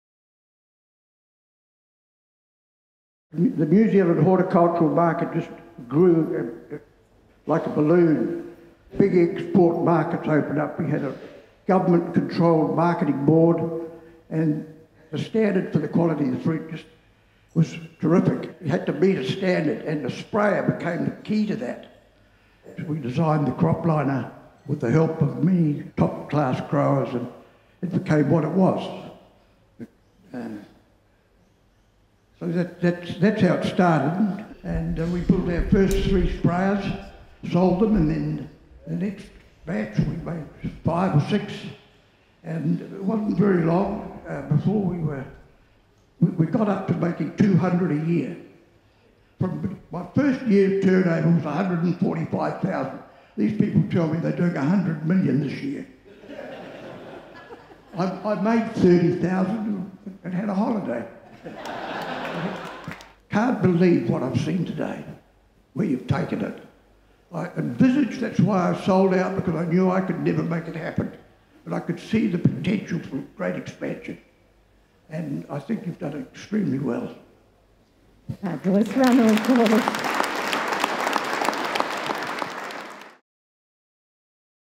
Croplands 50 Years | Miles Deck
Croplands Equipment celebrated 50 years of operations in 2022. Hear from from Croplands founder, Miles Deck on how it all began and what it's meant to him.